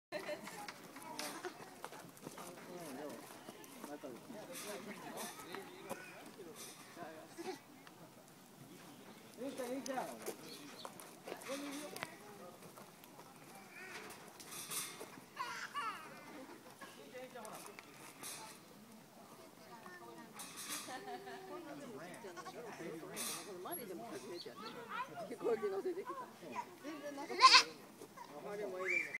cute goat
This goat was making a lot of noise when we walked away--I think he wanted more attention. Of course, once I turned on my video camera, he got quiet. Isn't that the way?